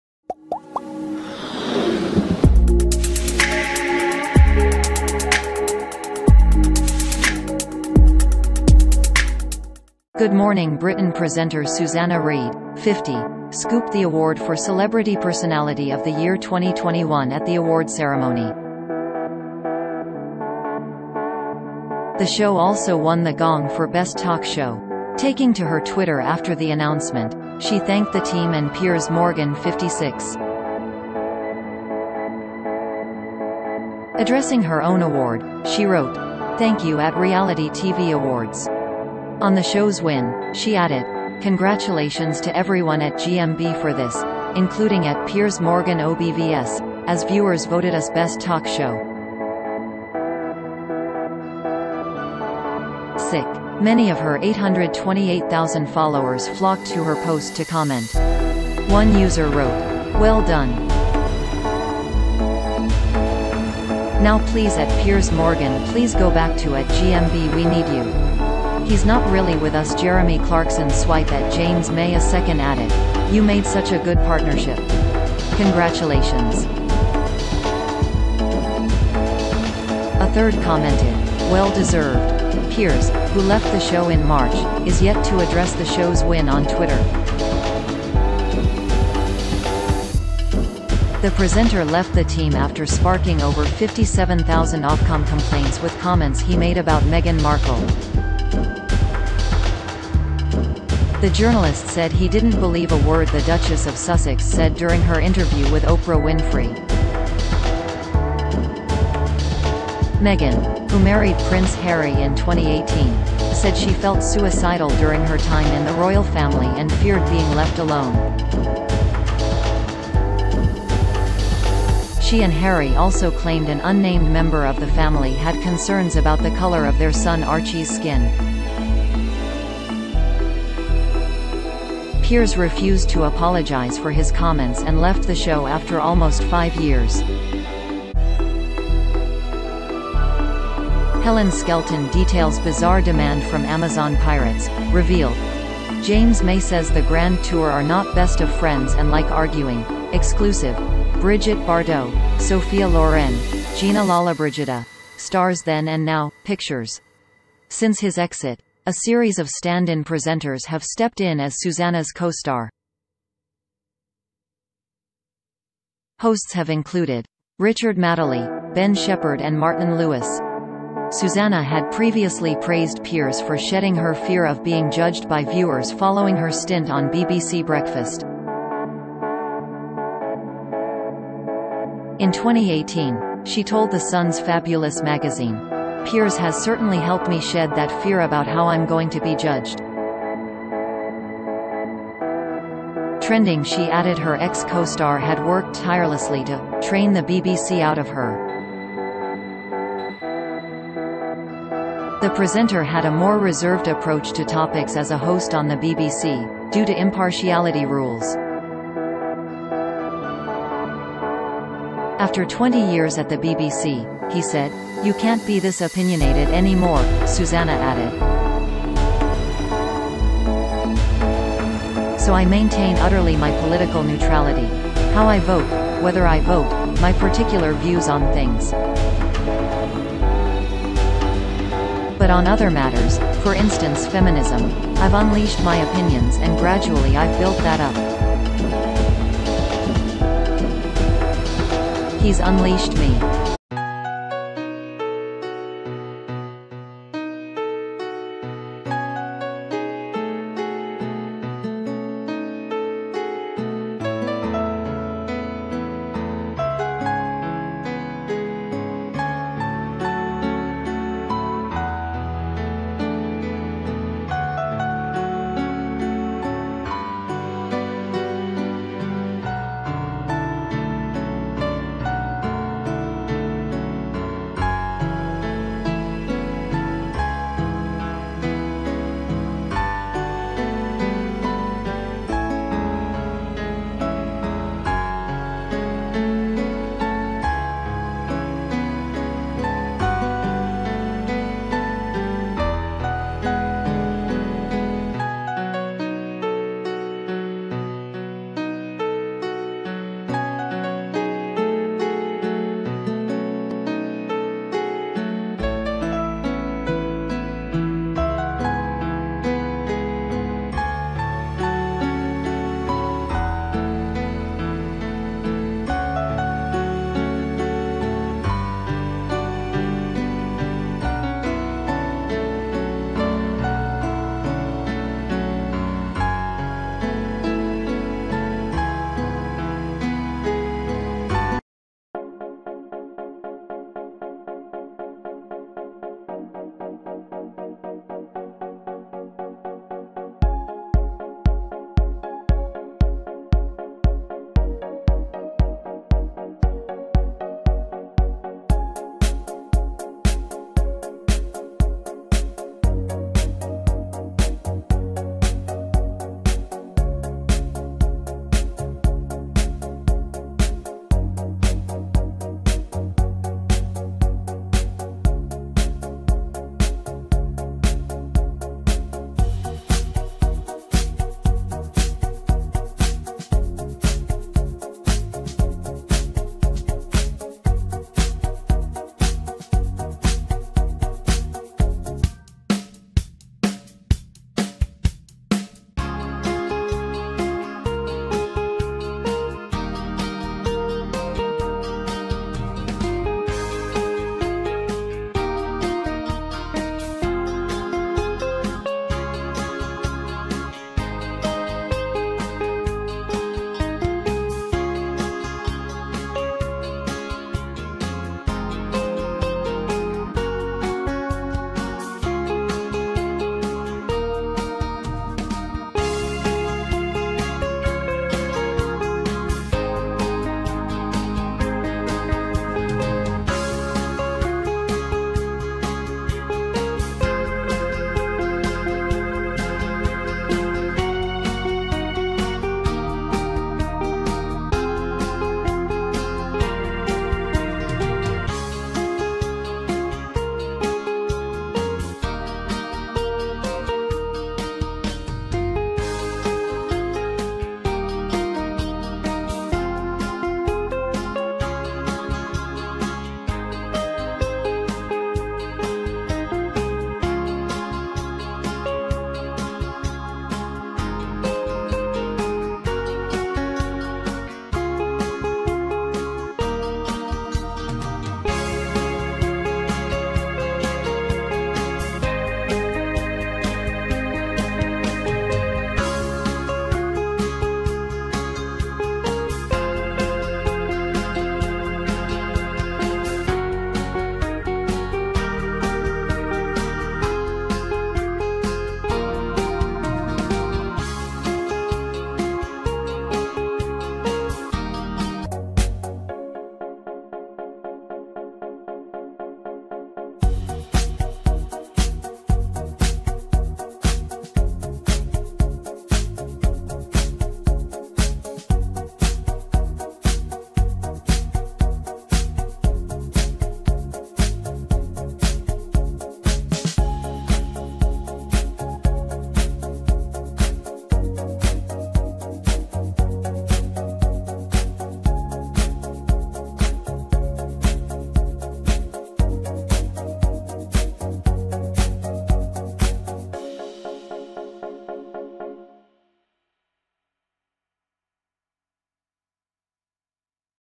Susanna Reid references ex co-star Piers Morgan as she scoops award for ITV's GMB
Good Morning Britain presenter Susanna Reid, 50, scooped the award for Celebrity Personality of the Year 2021 at the award ceremony. The show also won the gong for Best Talk Show. Taking to her Twitter after the announcement, she thanked the team and Piers Morgan 56. Addressing her own award, she wrote: “Thank you @RealityTVAwards.” On the show's win, she added: "Congratulations to everyone @gmb for this - including @piersmorgan obvs - as viewers voted us Best Talk Show."(sic) Many of her 828,0  ...